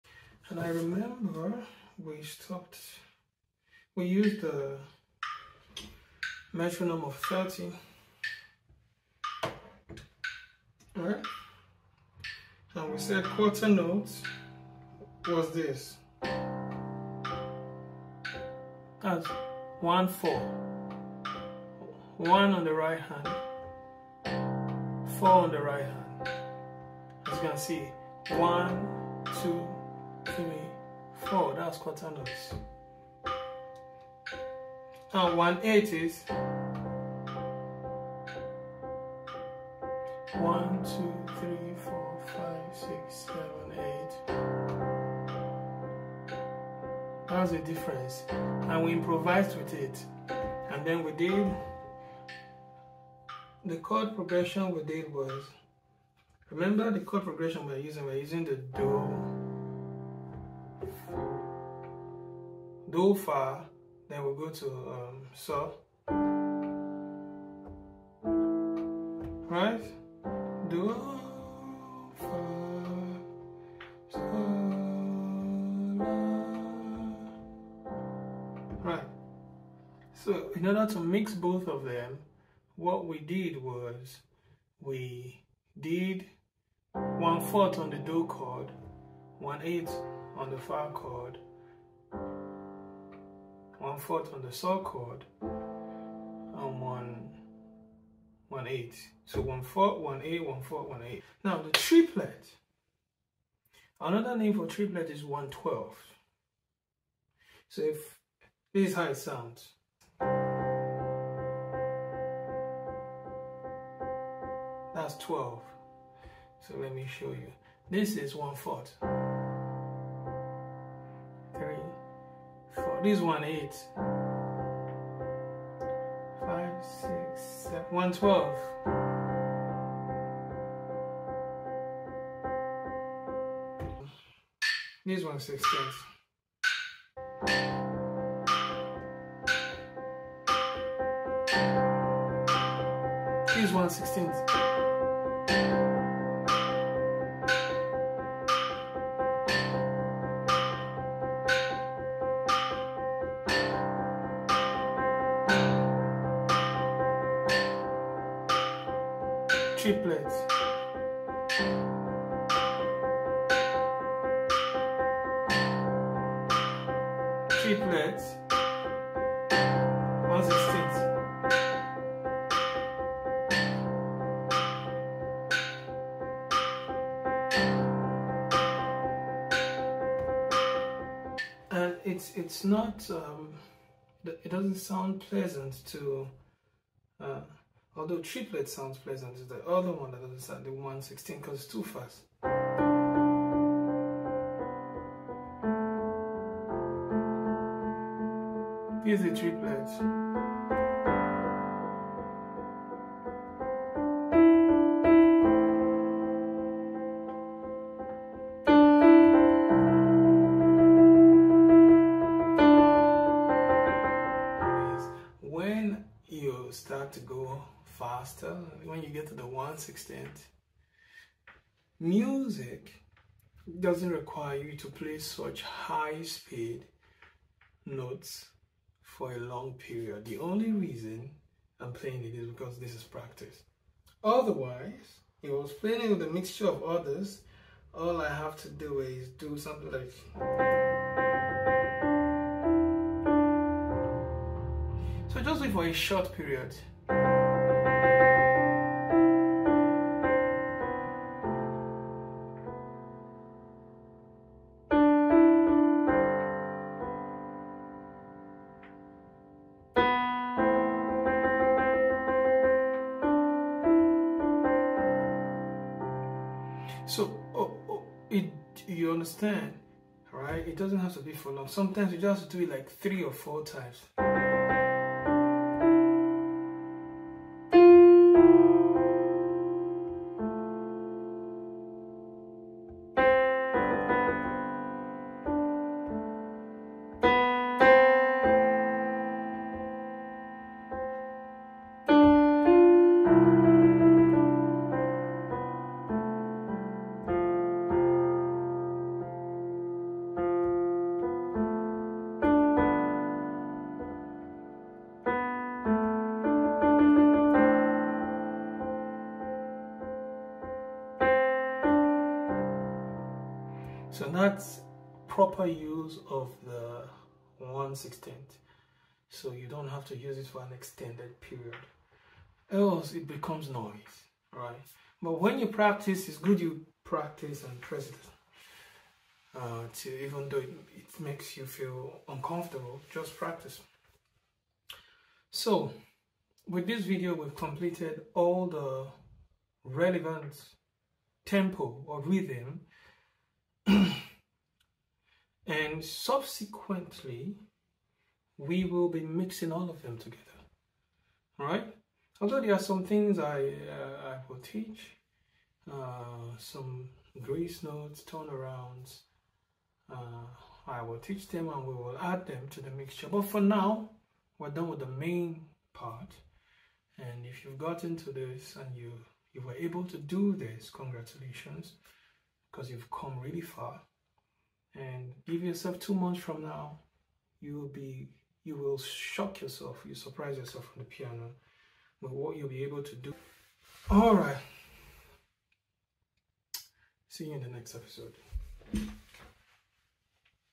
Episode 23 | Right Hand Improvisation Part 5 | Key C | Intermediate level
Welcome to Episode 23 of our Intermediate Piano Improvisation Series! In this installment, we dive into the world of right-hand piano improvisation in the key of C. Join us as we explore the fascinating realms of 1/12th aka Triplet and 1/16th tempo improvisation, taking your piano skills to the next level. Whether you're a budding pianist or looking to enhance your musical creativity, this episode has something for everyone. Let's unleash the power of your right hand on the keys!